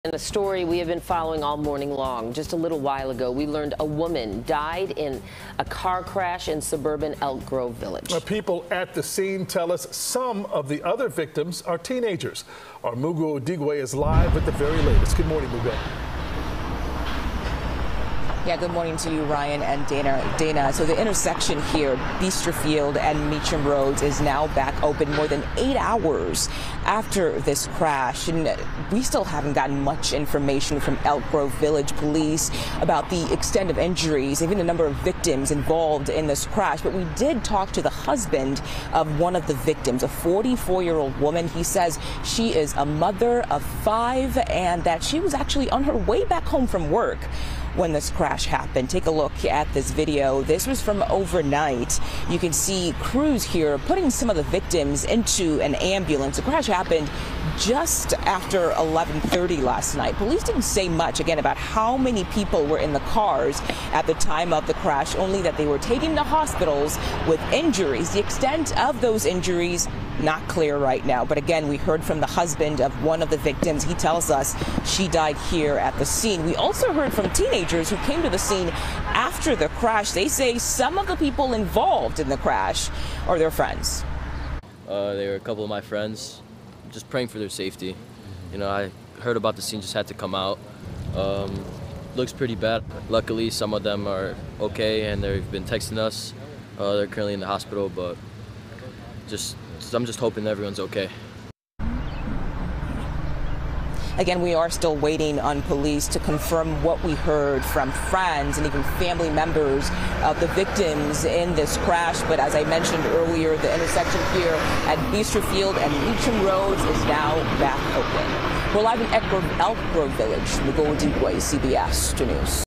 One killed, multiple people hospitalized after serious crash in Chicago suburb
A woman was killed, and multiple people were sent to local hospitals after a serious crash in suburban Elk Grove Village Tuesday night. Family members at the scene said a 44-year-old woman died in the crash on her way home from work. Authorities have not confirmed any deaths or the extent of the injuries.